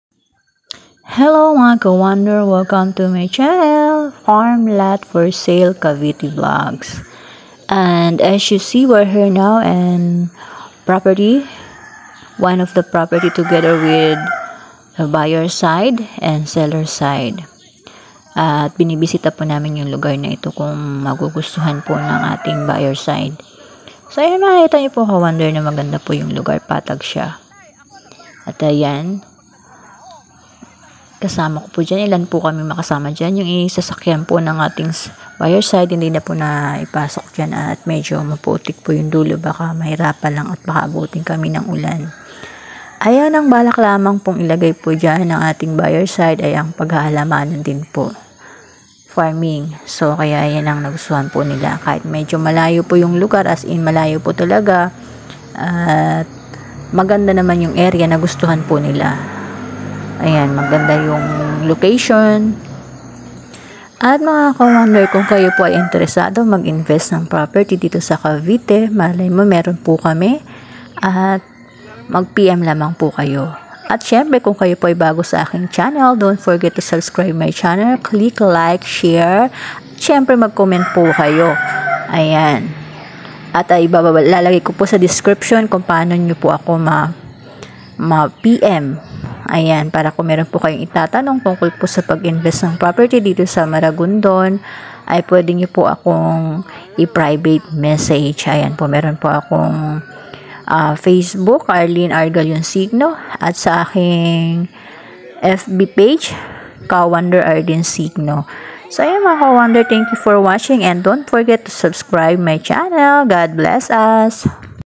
PAGBISITA NG BUYER SIDE SA 10 HECTARES MARAGONDON CAVITE
Hello mga Ka Wonder! Welcome to my Channel Sa video ko pong ito ay nandito po lami sa isang Property for sale na may kabuuang sukat na 10 hectares.Kasama kopo dito ang buyer side na binista ang lupa.At makikita nyo po na maganda ang location patag at ilalagay po diyan ng makakabili ay mag fa farming din at mag hahalamanan.Inaangkin napo namin na madeal po namin ito.

For More Information about Property for Sale just add my..

FACEBOOK ARLENE  ARGALLON SIGNO
FB PAGE KA WONDER ARLENE SIGNO